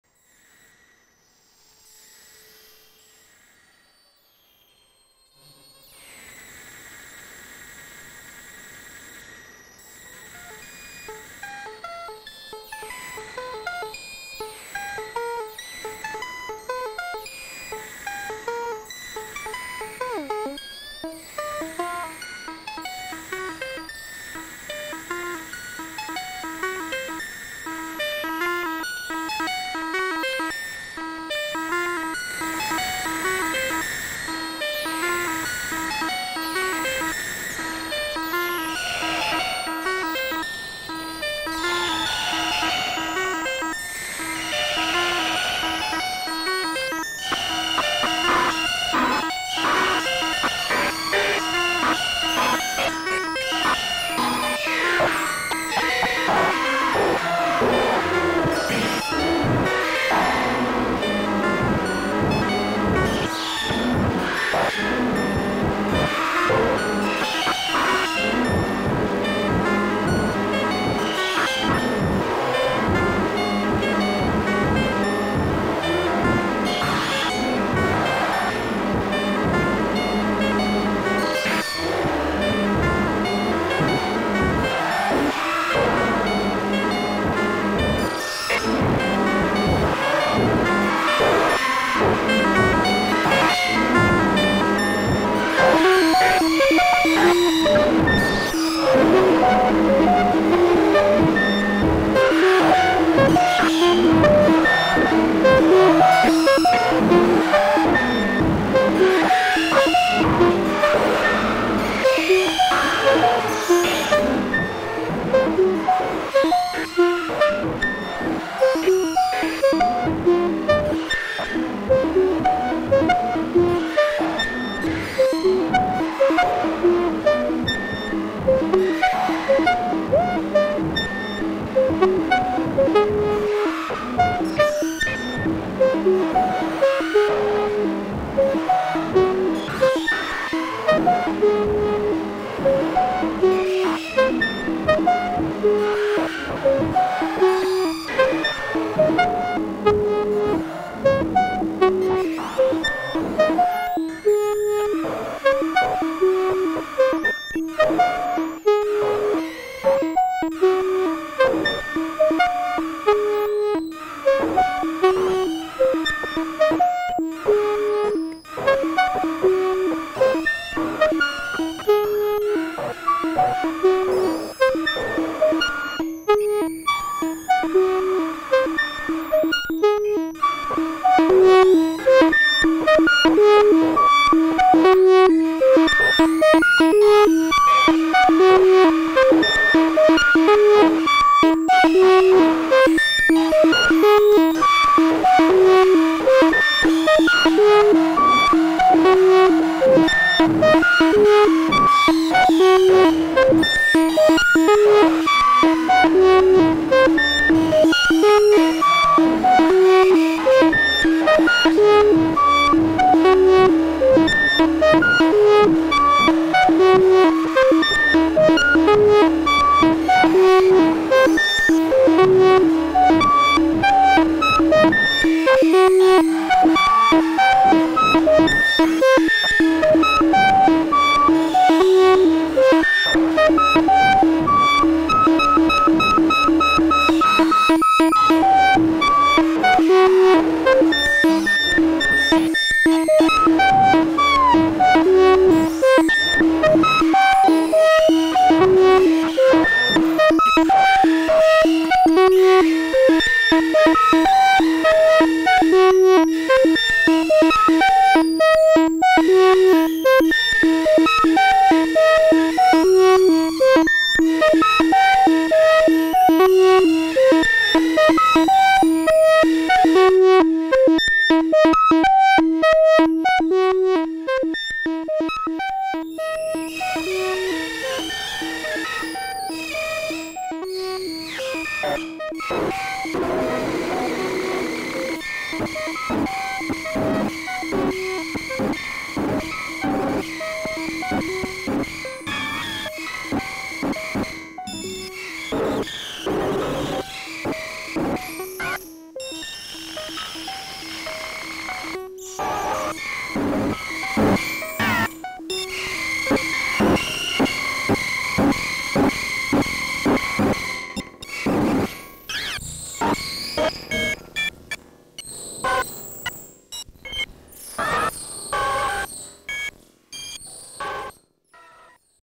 Shattered Signal Experimental Modular Jam FEAT Make Noise 0-coast, 0-cntrl, Strega, Instruo Scion
More experimental modular stuff, unfortunately the live vid got a bit borked so I couldn't upload that